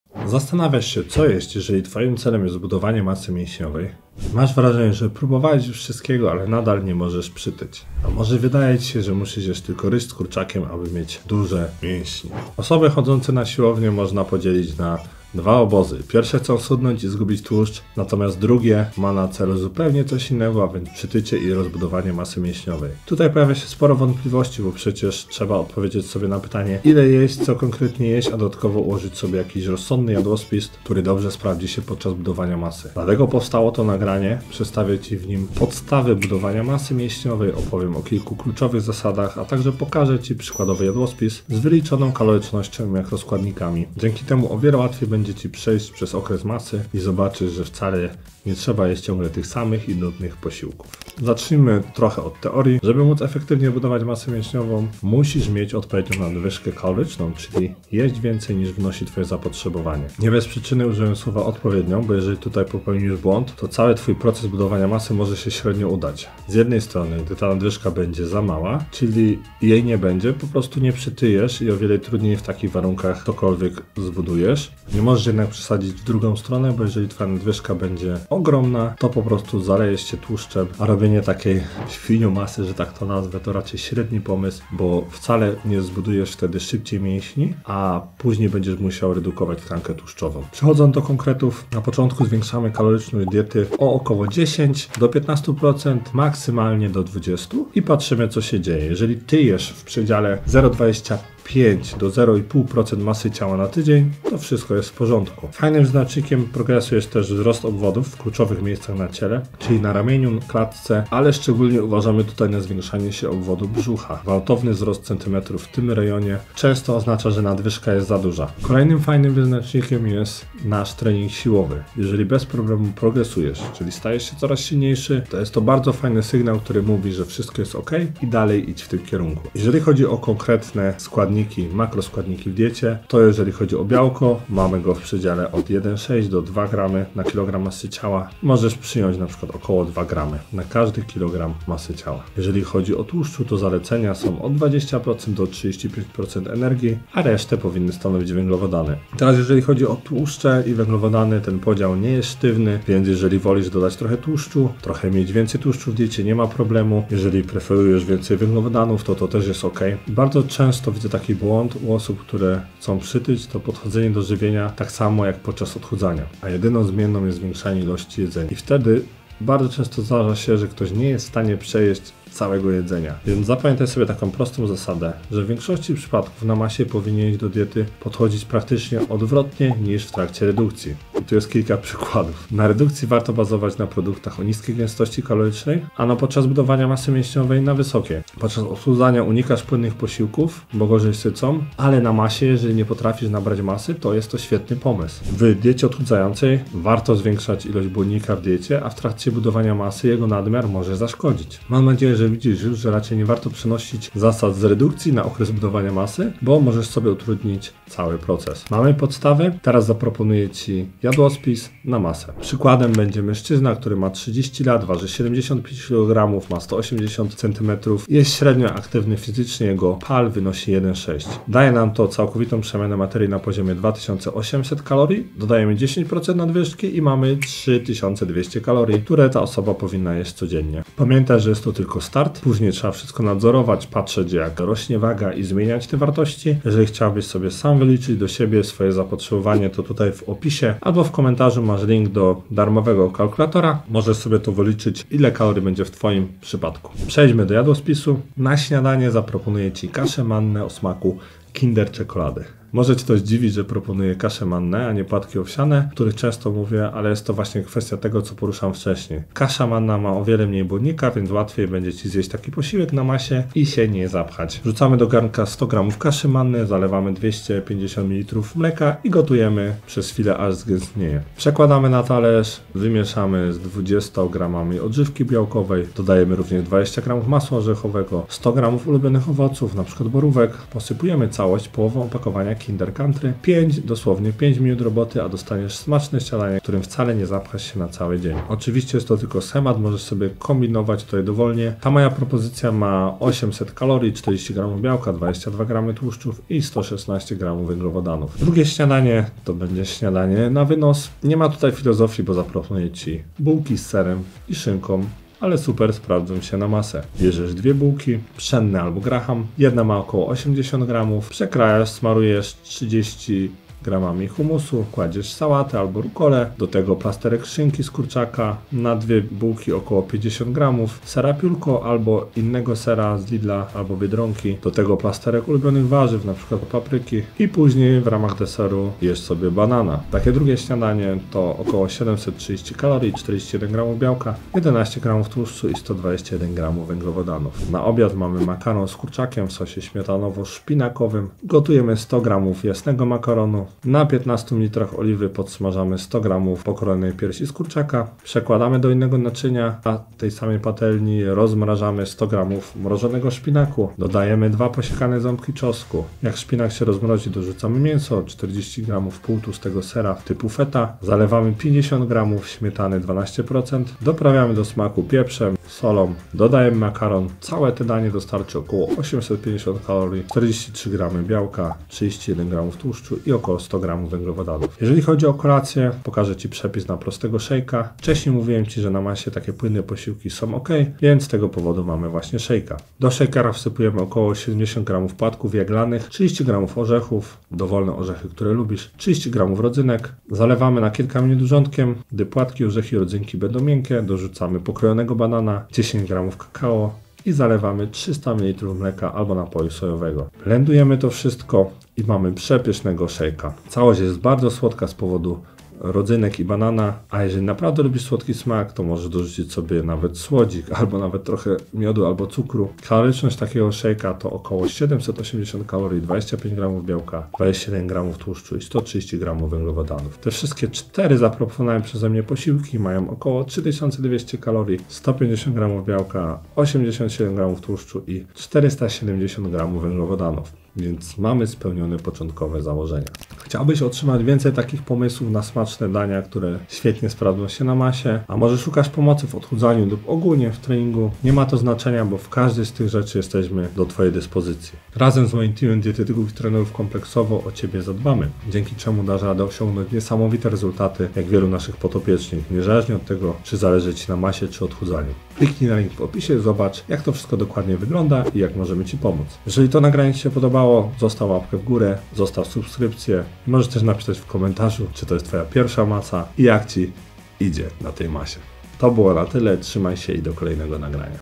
Najlepsza dieta na masę – co jeść, żeby przytyć i zbudować masę mięśniową? Trener Mariusz Mróz
Dzisiaj przygotowałem dla Ciebie film Najlepsza dieta na masę – co jeść, żeby przytyć i zbudować masę mięśniową? Przedstawię Ci jadłospis, abyś mógł przybrać na masie – sprawdzi się on idealnie dla wszystkich początkujących i dla każdego chudzielca! Moje pomysły na jedzenie na pewno Cię zaskoczą, a dodatkowo będzie to tania propozycja. Budowanie masy i mięśni nigdy nie było tak proste. Koniecznie oglądaj do końca, Trener Mariusz Mróz.

ROZDZIAŁY:

0:00 Najlepsza dieta na masę – co jeść, żeby przytyć i zbudować masę mięśniową?
0:50 Ile kalorii jeść na masę?
1:38 Jak mierzyć progres podczas budowania mięśni?
2:10 Rozkład makroskładników na masie
2:40 Jak podejść do żywienia w trakcie budowy masy?
4:07 Śniadanie, które warto jeść aby przybrać na masie
4:58 Kolejny posiłek w jadłospisie na budowę mięśni
5:37 Idealny obiad na masę dla chudzielca
6:14 Kolacja w diecie na masę dla początkujących